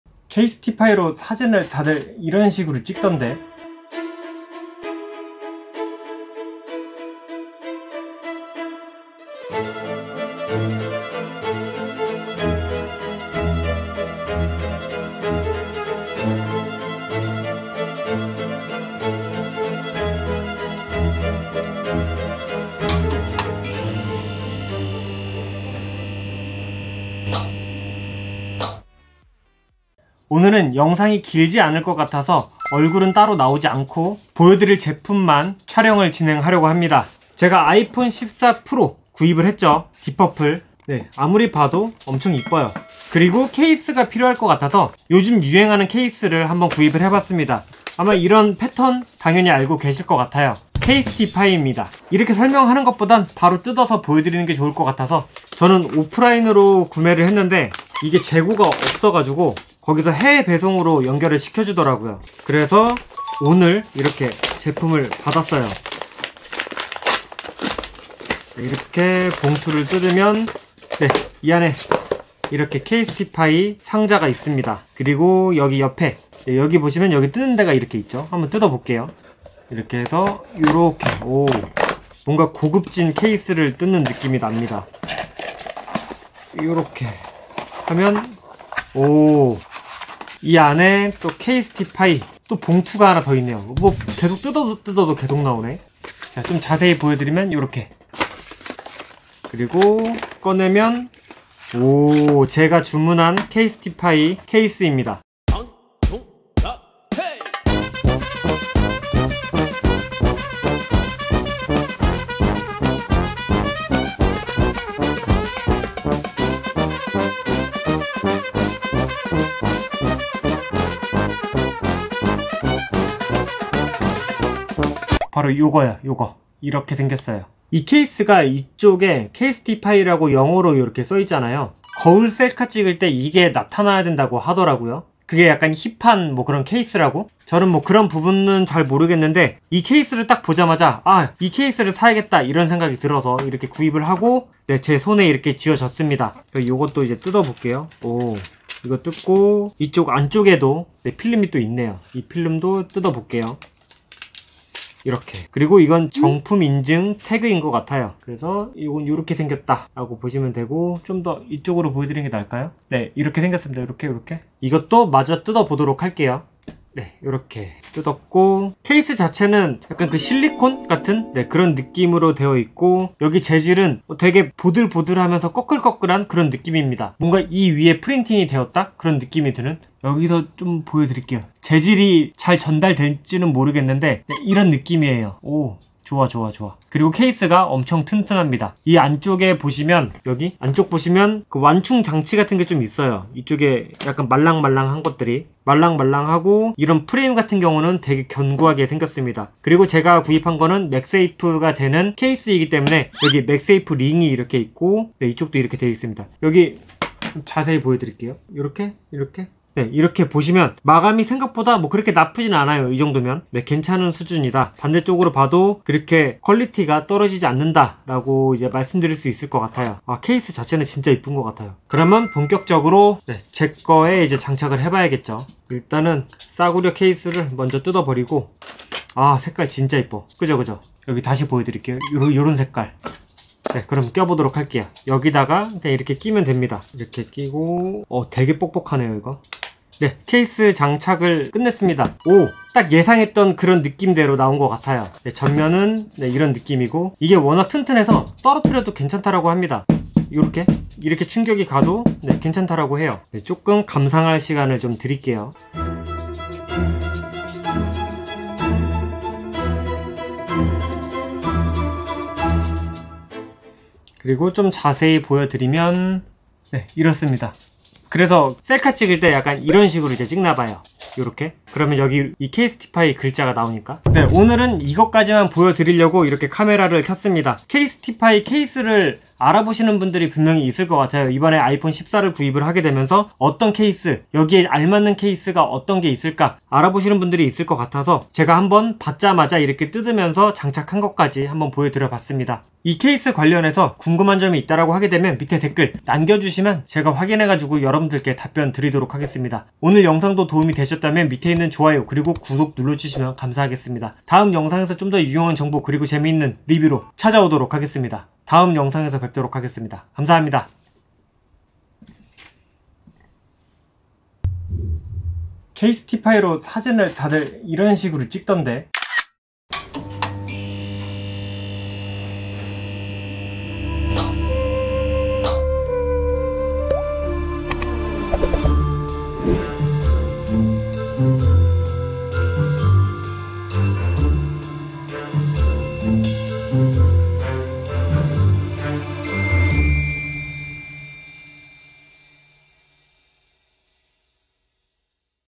(4K)📱[리뷰] 케이스티파이 아이폰14프로 맥세이프 전용 케이스 언박싱 리뷰 | casetify 힙한케이스 셀카케이스 거울셀카 재활용케이스
아이폰14프로를 구입하고, 바로 이어서 케이스를 하나 구입을 했습니다.
마침 근처에 케이스티파이 오프라인매장이 있어서 거기에서 구입을 하고,
(재고가 없어서) 해외배송으로 따끈따끈하게 받아보았습니다!
언박싱부터 장착까지 여러분들께 보여드리고 싶어서 짧게 영상으로 남겨봅니다

아이폰14프로를 쿠팡에서 할인받고 로켓배송으로 받아보세요!